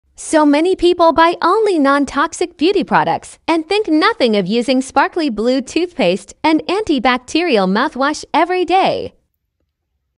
Let's talk about "nontoxic" dental products...
Toothpaste is regulated as a cosmetic product… but toothpaste is put IN OUR BODIES